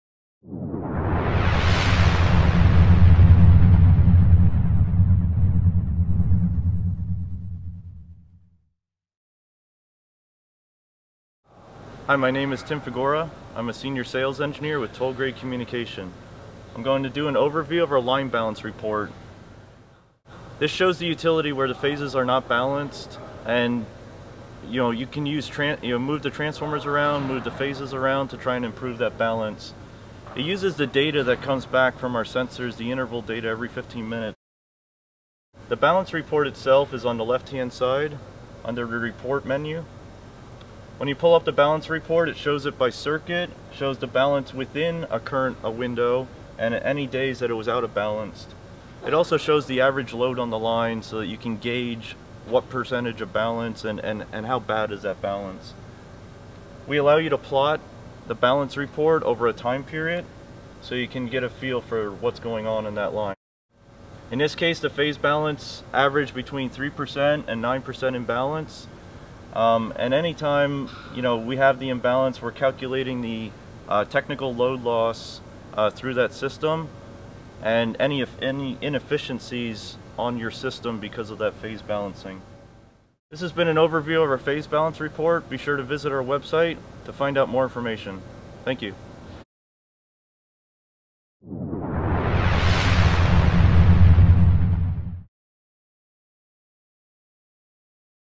Phase balance – How to detect power line phase imbalance?
Wach this overview of the LightHouse platform auto-phase identification feature. Learn how LightHouse helps you identify which phases are not balanced and how it can help you improve it.